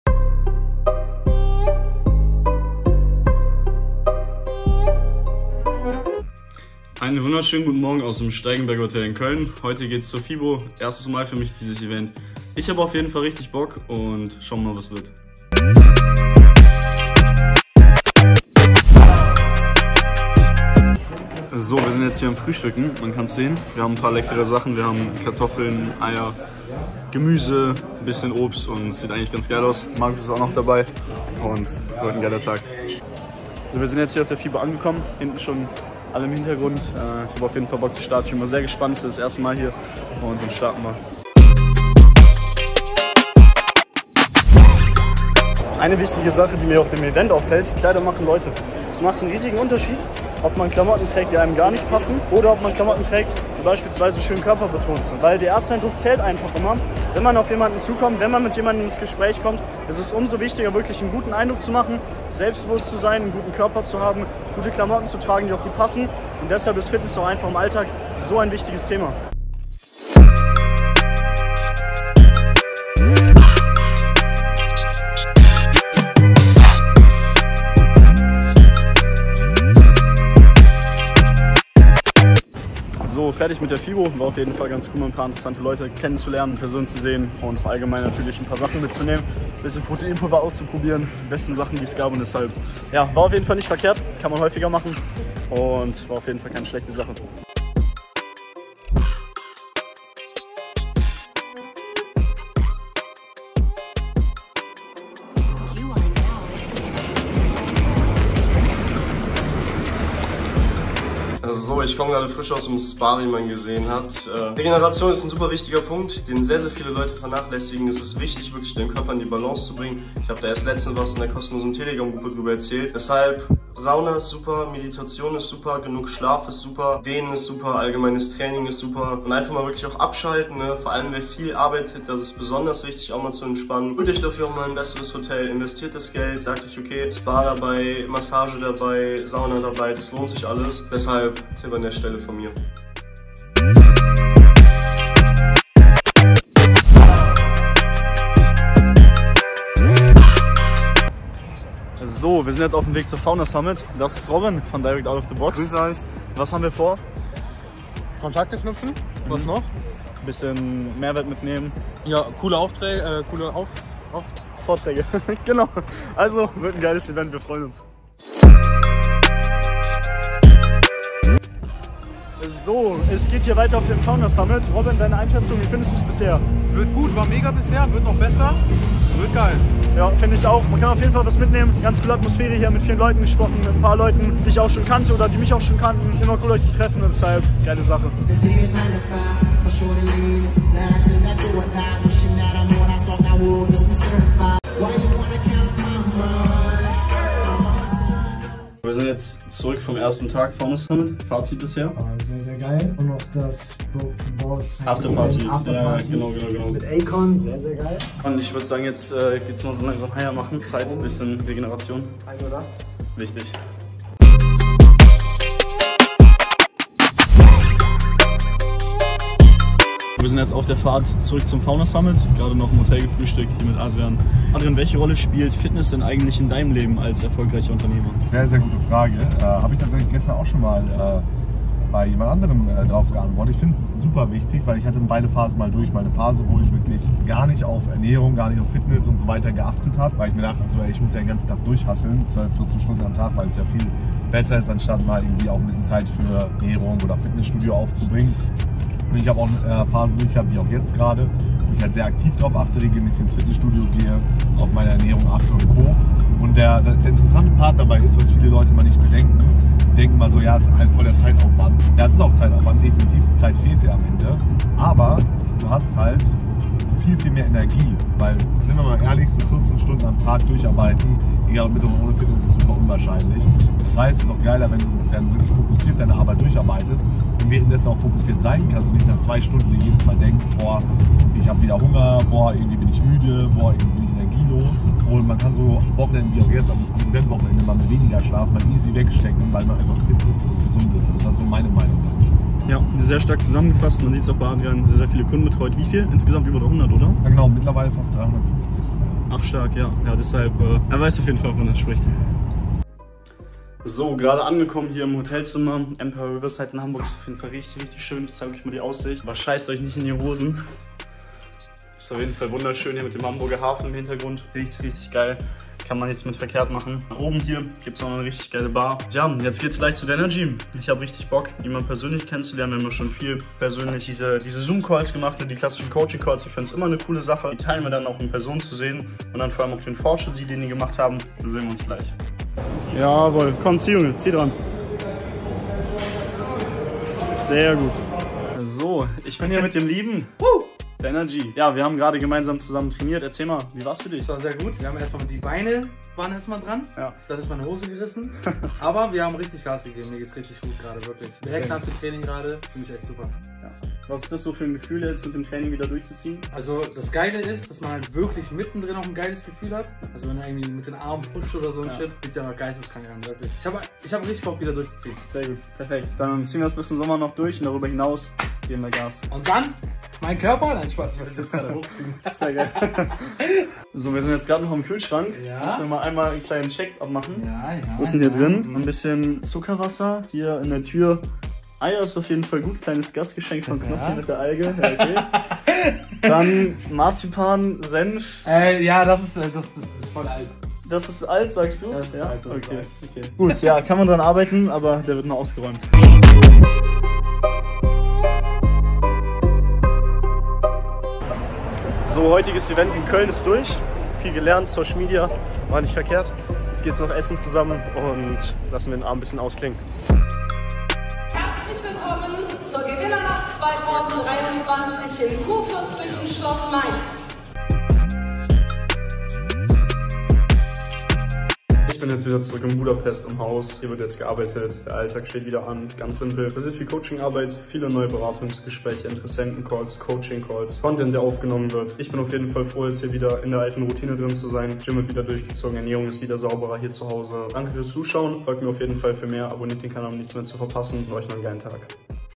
FABIAN PRIETZ Vlog - Training mit DANERGY, Events und FITNESSTIPPS auf Reisen
Mitte April war ich für zwei Wochen in Deutschland unterwegs. Im Video seht ihr wen ich getroffen hab, was alles passiert ist und natürlich eine Menge Tipps für euch.